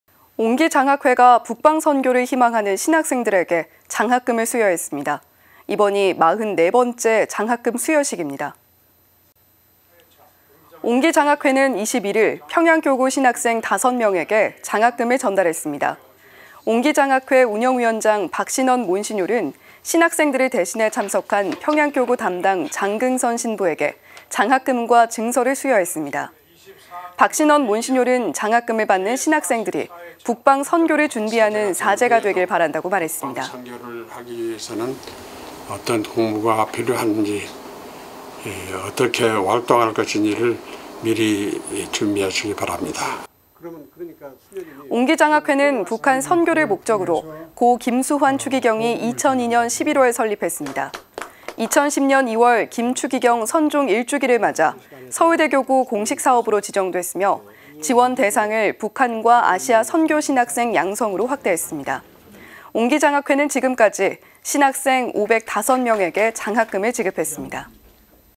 옹기장학회 올해 2학기 장학금 수여…신학생 5명 수혜
옹기장학회가 북방선교를 희망하는 신학생들에게 장학금을 수여했습니다.

이번이 44번째 장학금 수여식입니다.

후원문의, 매일미사 지향 신청 : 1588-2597
카카오톡 플러스친구 : cpbc TV